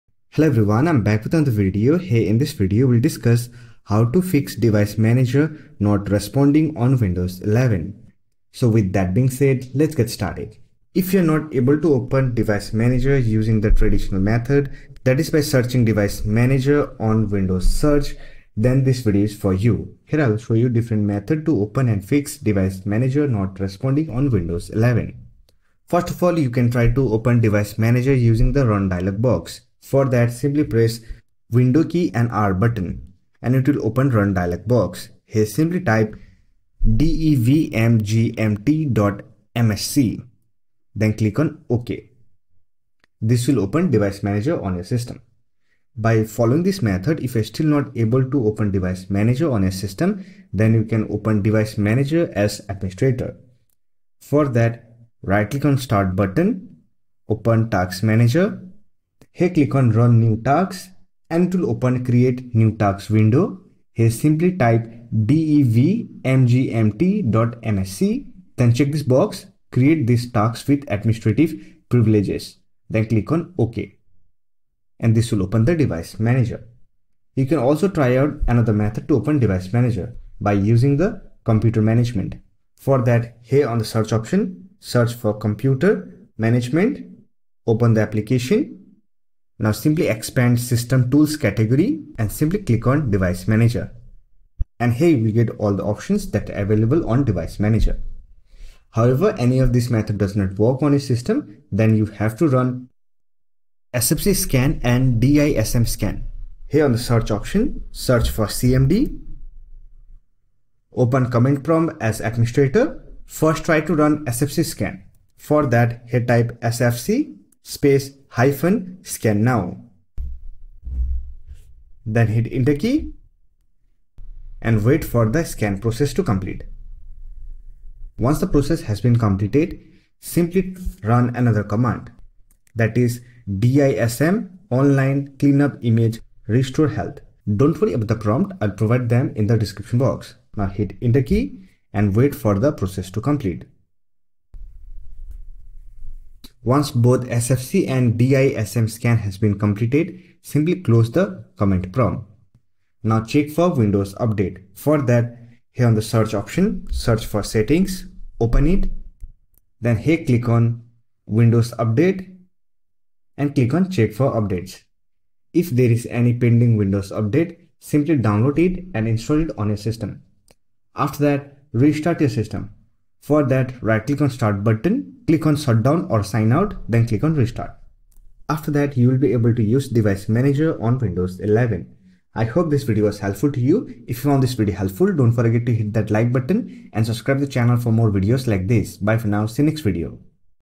How to Fix Device Manager Not Responding on Windows 11
In this video, we are going to learn how to fix the Device Manager not responding on Windows 11. Device Manager is where you can manage all your connected device drivers. However, sometimes it might not respond. To fix this, follow these troubleshooting methods.